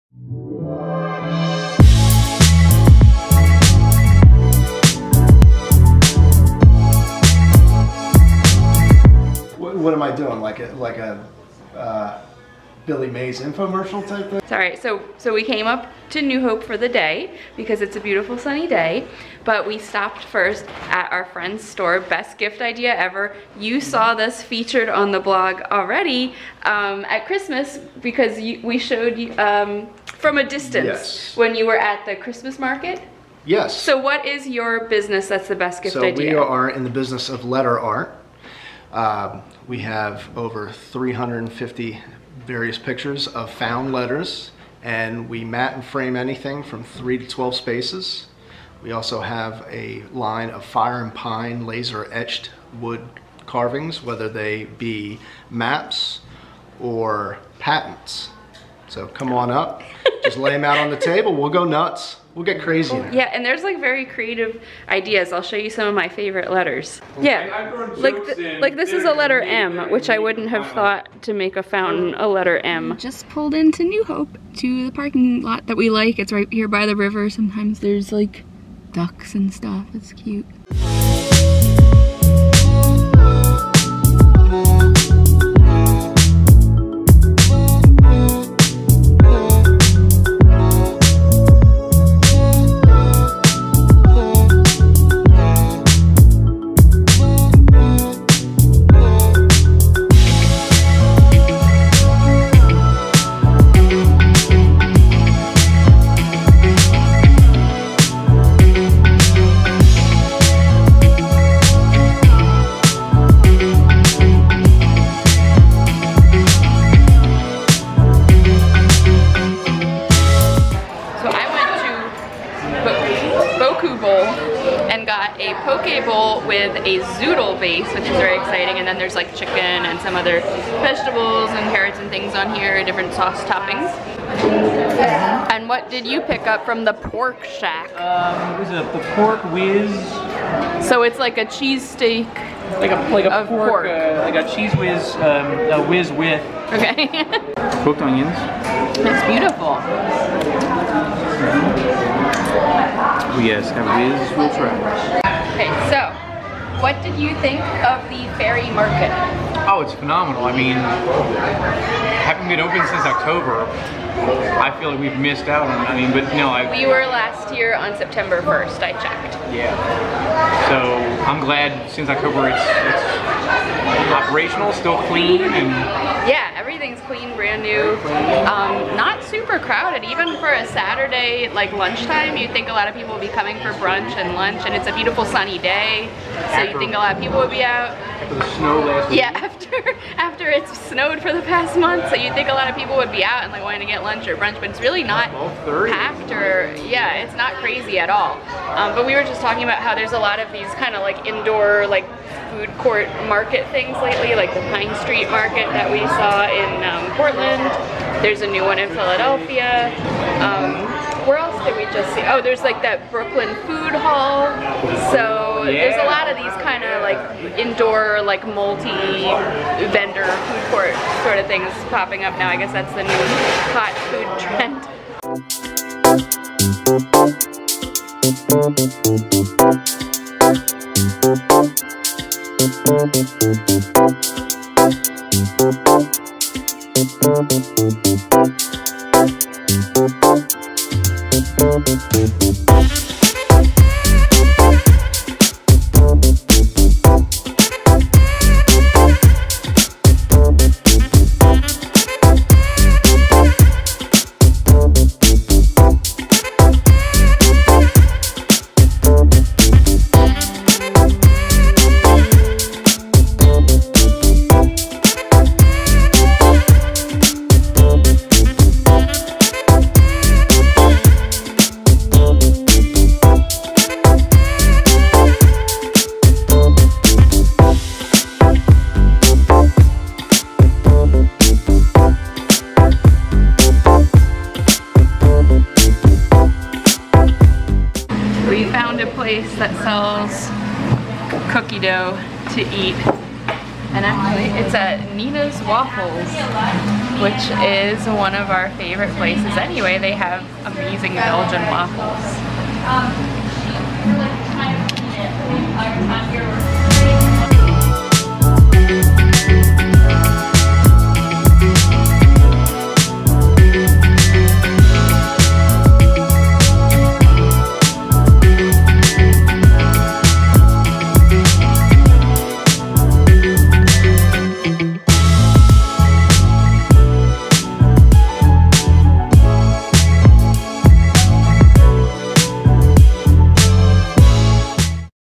SATURDAY IN NEW HOPE | Poke Bowl, Cookie Dough, Bookstore Cat 🐈
GG and I made a springtime trip up to New Hope, PA on the first sunny day we had in a long time! Start with a visit to Best Gift Idea Ever in Peddler's Village lunch at the new Ferry Market from Boku Bowls and Pork Shack and tea from Skyroast Coffee a visit with Butter the cat at Farley's Bookshop and cookie dough at Nina's Waffles - definitely not keto friendly!